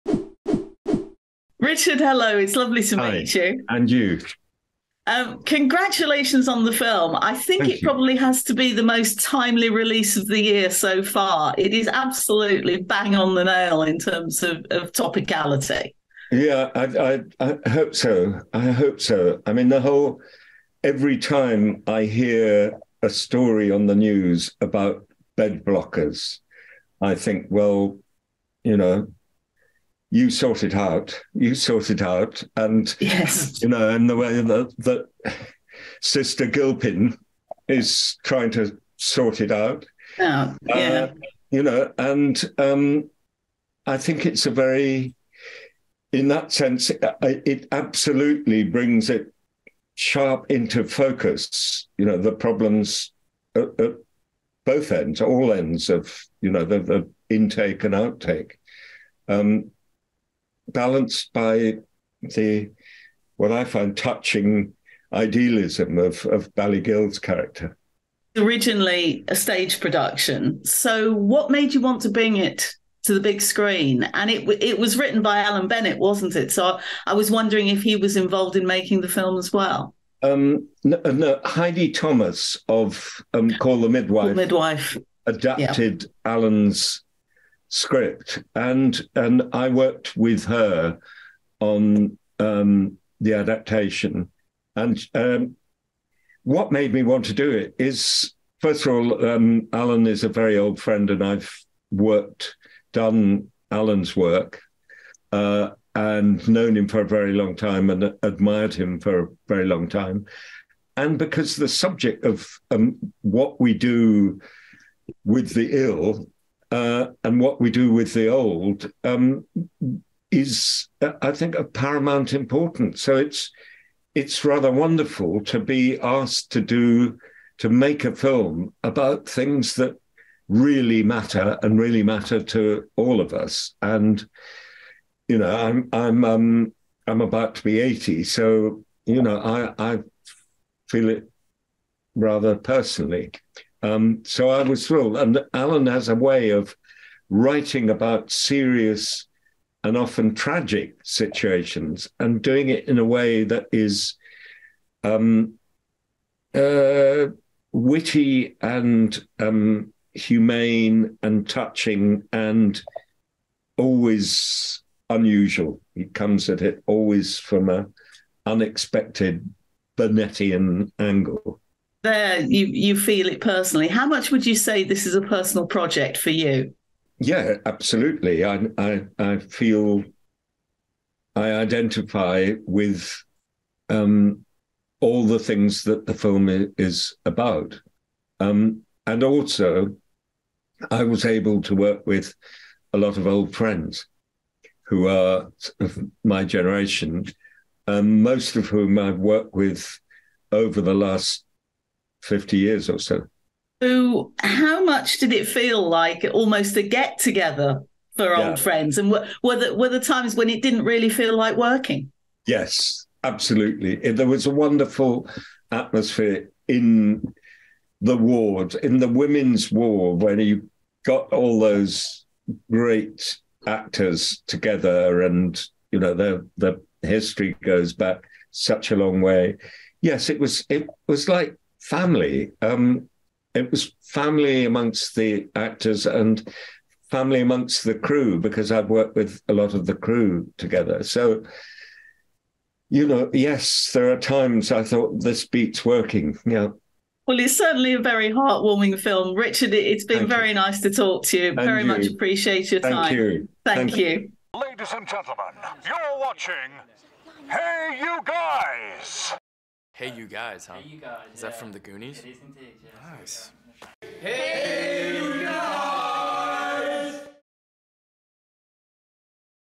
Allelujah - Richard Eyre on the heartwarming, serious message of the film & adapting Alan Bennett
Richard Eyre is interviewed for Allelujah. Based on the stage play by Alan Bennett and directed by Richard Eyre, the film stars Jennifer Saunders, Bally Gill, Judi Dench, Derek Jacobi, David Bradley, Russell Tovey and Julia McKenzie. Freda Cooper asks the questions.

Plot: When the geriatric ward in a small Yorkshire hospital is threatened with closure, the hospital decides to fight back by galvanizing the local community: they invite a news crew to film their preparations for a concert in honour of the hospital’s most distinguished nurse. The story celebrates the spirit of the elderly patients whilst paying tribute to the deep humanity of the medical staff battling with limited resources and ever-growing demand.

‘Allelujah’ is set to open in cinemas across the UK and Ireland on 17 March 2023.